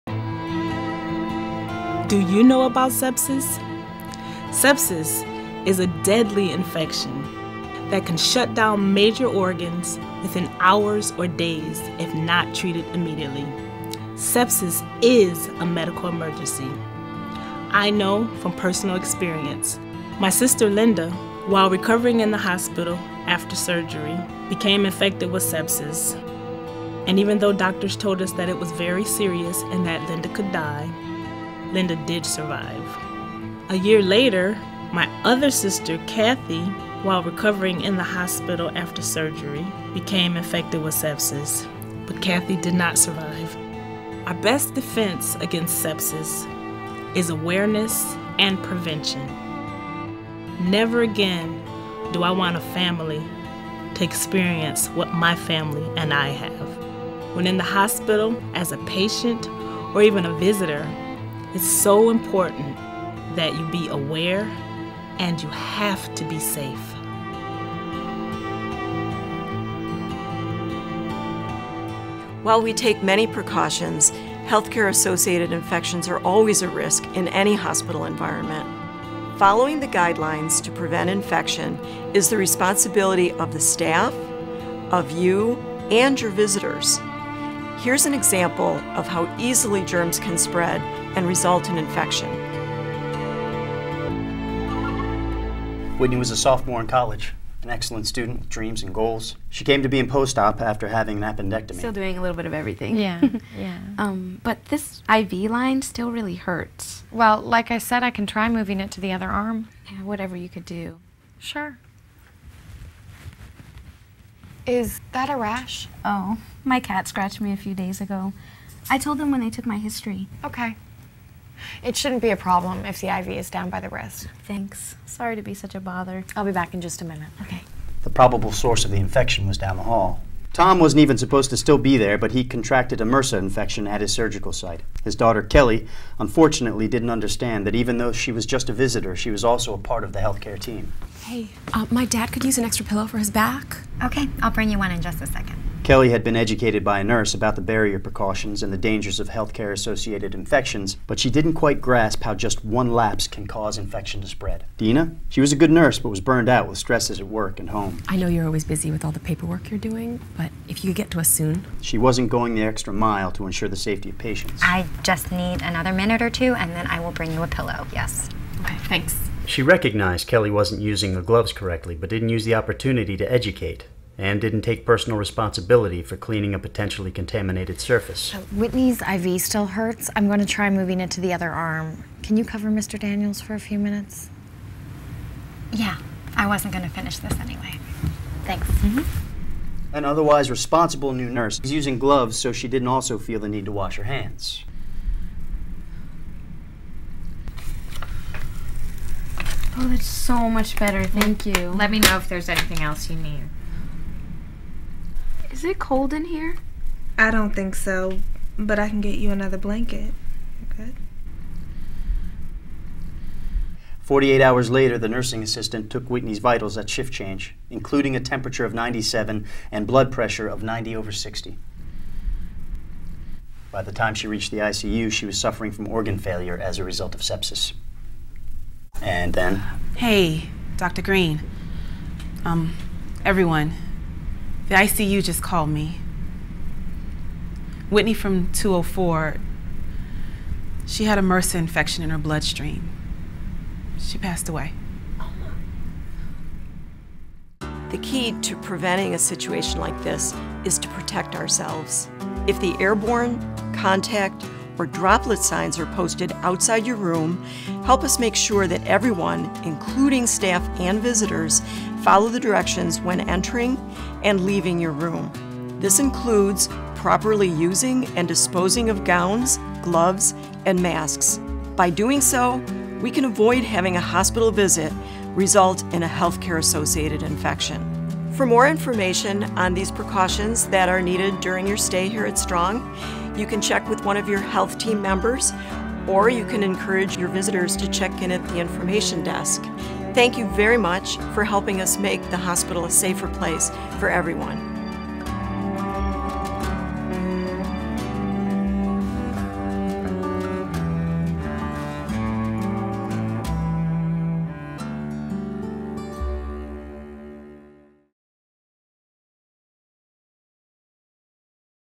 Infection Prevention is Everyone's Business
The term "infection prevention" represents practices which are employed and recommended by healthcare providers and your family to prevent the spread of germs.

Learn more about the University of Rochester's infection prevention procedure